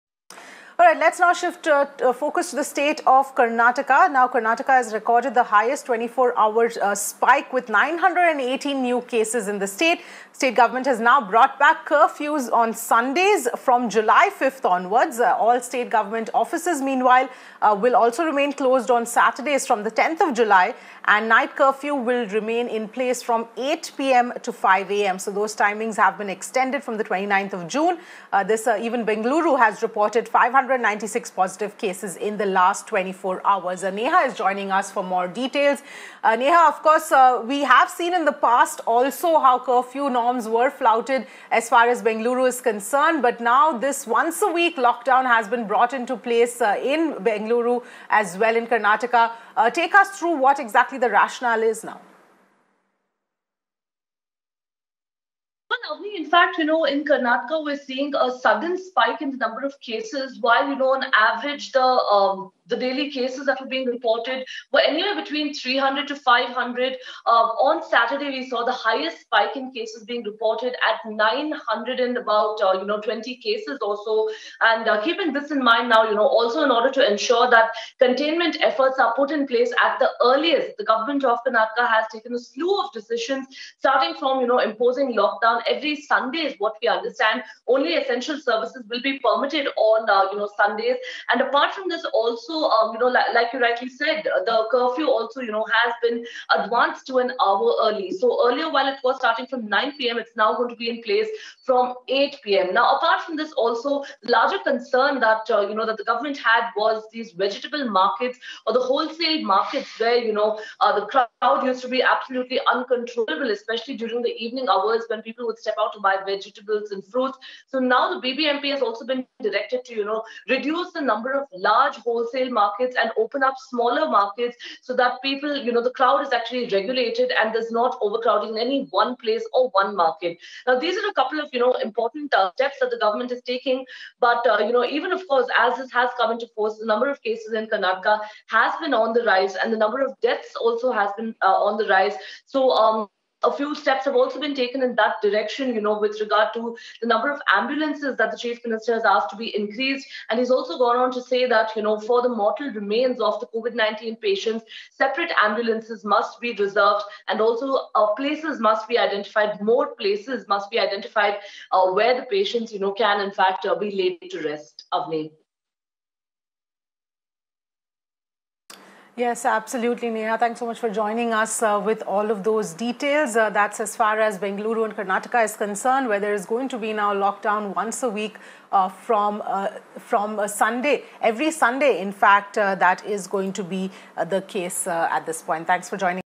Karnataka reimposes lock-down restrictions due to surge in cases
The surge in the cases of the COVID-19 cases has forced the Karnataka government to impose lockdown restrictions again the state. It recently has the highest spike in the past 24 hours with 918 cases. The restrictions would be similar to that of lockdown 2, wherein people can go out only for necessary essentials. Bengaluru saw more than 596 cases in the city. The people will not be allowed to go out for any meets on Sundays, as the state will undergo curfew. The lockdown on Sundays will begin on July 5. The recent surge is credited to the opening of the state and unregulated supervision. Watch the video to know more!